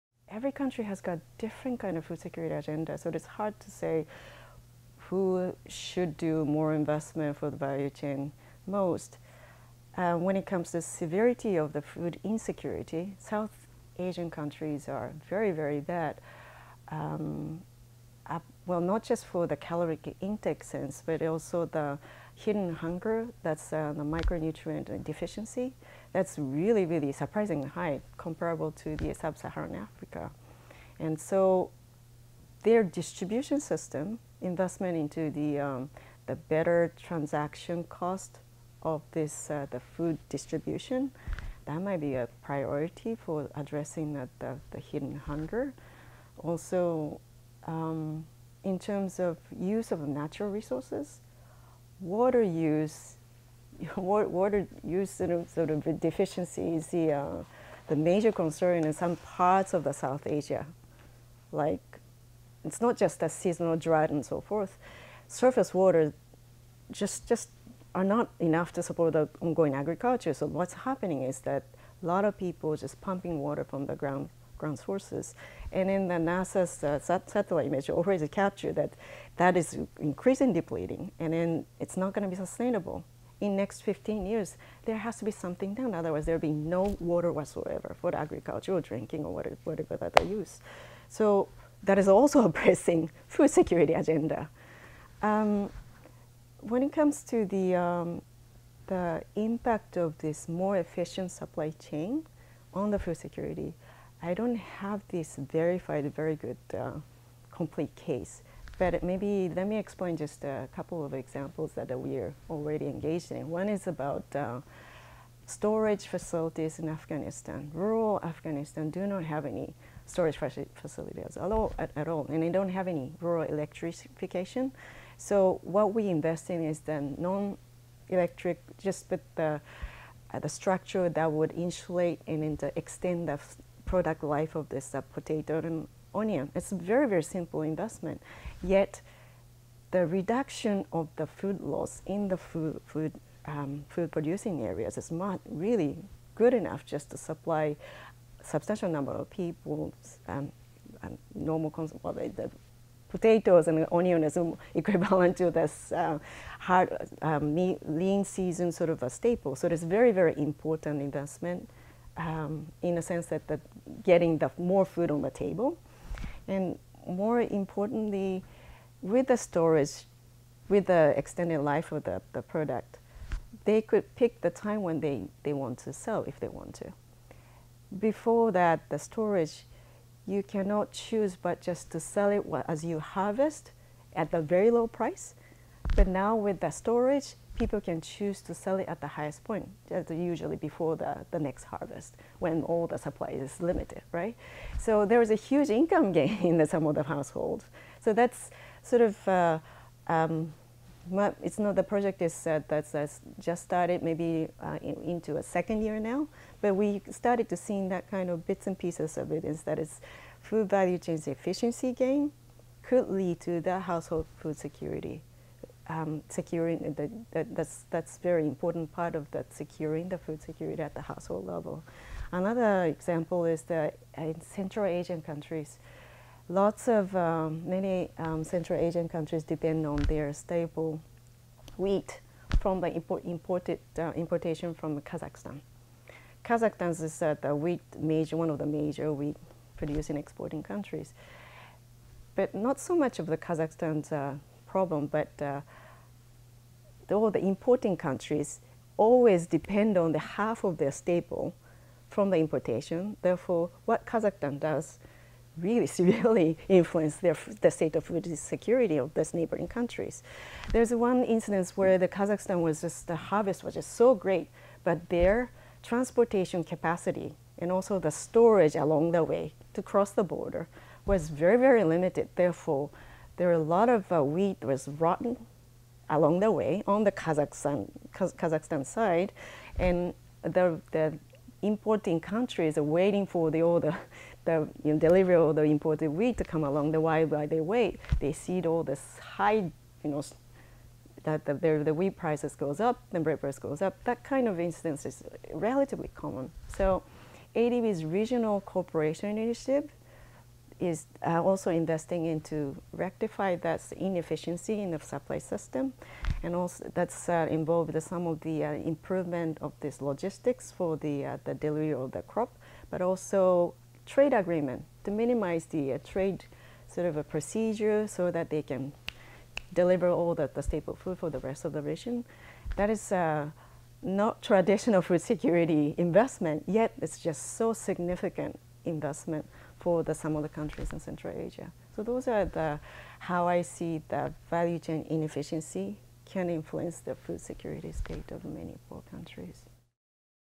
Want to feed Asia? Tackle inefficient supply chains (Part 3 of 4)
Food prices in Asia are expected to rise until 2050, in part due to inefficient supply chains. In this video interview for our Feeding Development campaign, an ADB expert discusses how a structural transformation of the whole distribution process can achieve food security.